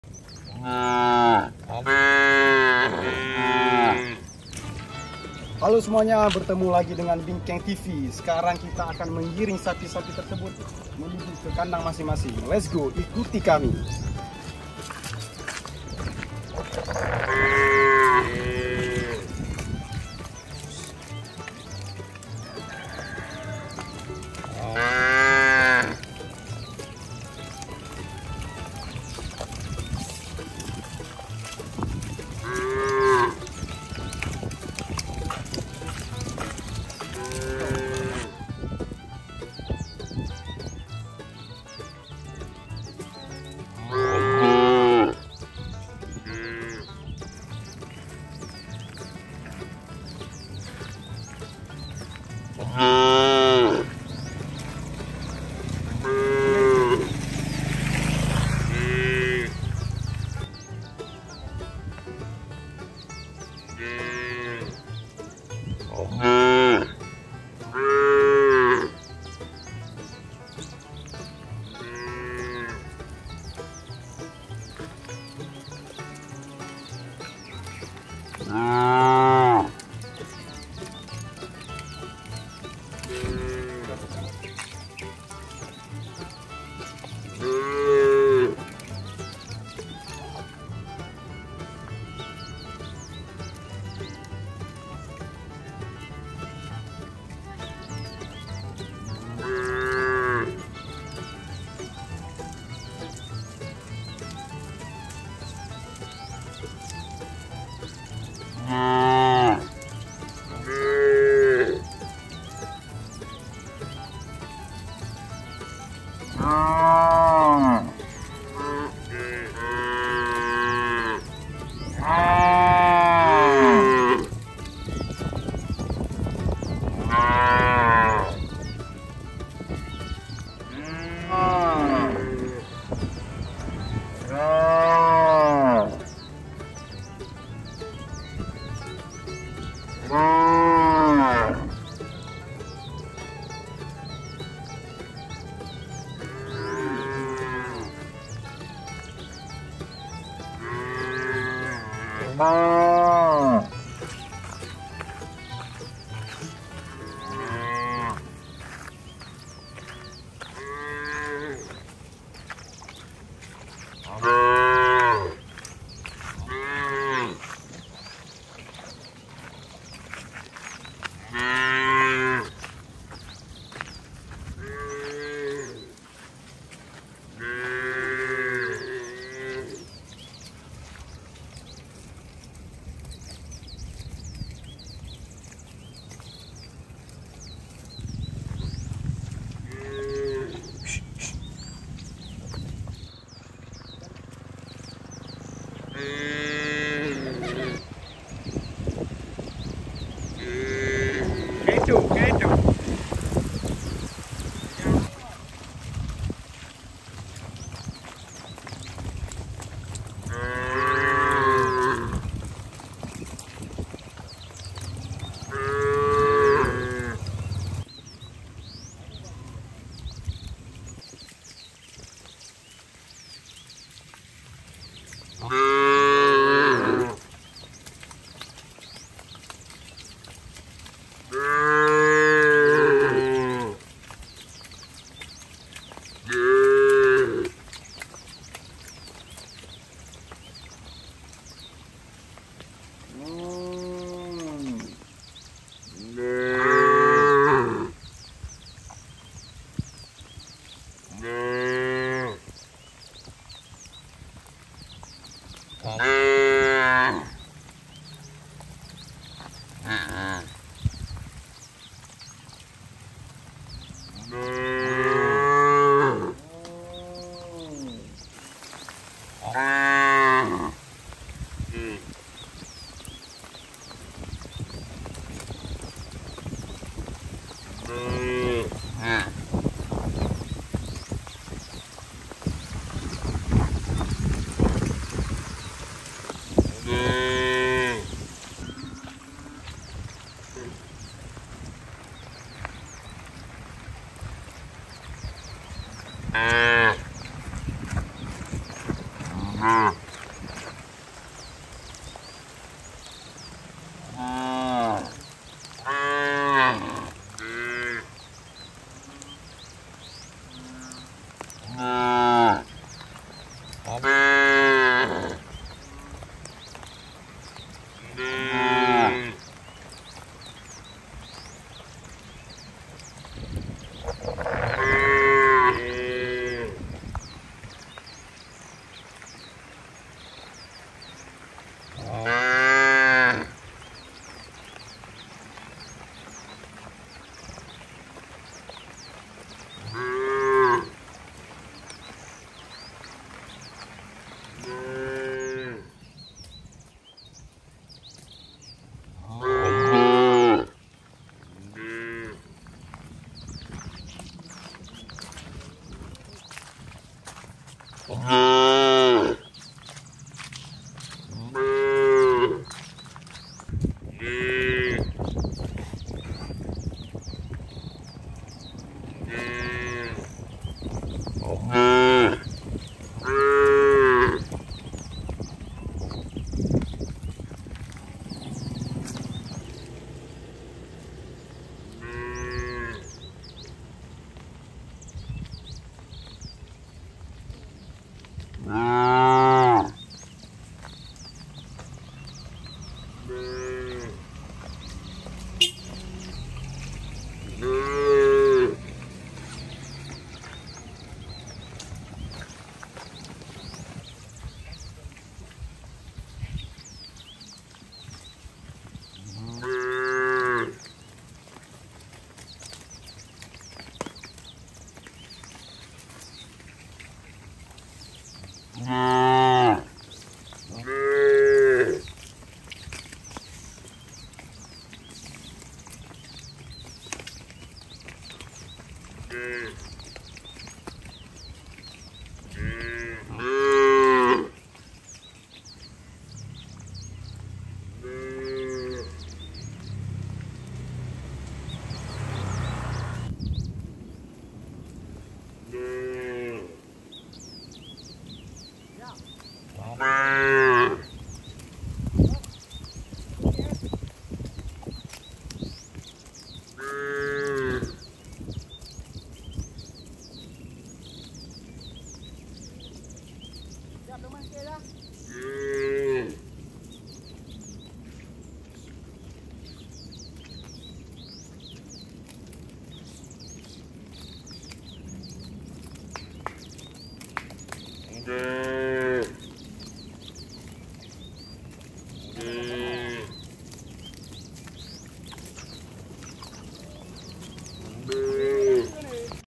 Video Menggiring Sapi Lembu Menuju Ke Kandang Peternakan Hewan Ternak pada musim Hujan - Bunyi sapi
Video ini menunjukkan kegiatan menggiring sapi menuju kandang peternakan saat musim hujan. Dalam cuplikan ini, sapi-sapi tampak bergerak perlahan di jalanan basah, dan suara mereka yang khas bisa terdengar di latar belakang. Musim hujan menambah tantangan dalam proses ini, dengan jalanan yang licin dan cuaca yang lembap. Video ini memberikan gambaran tentang rutinitas peternakan serta kondisi yang dihadapi oleh peternak dalam mengelola hewan ternaknya.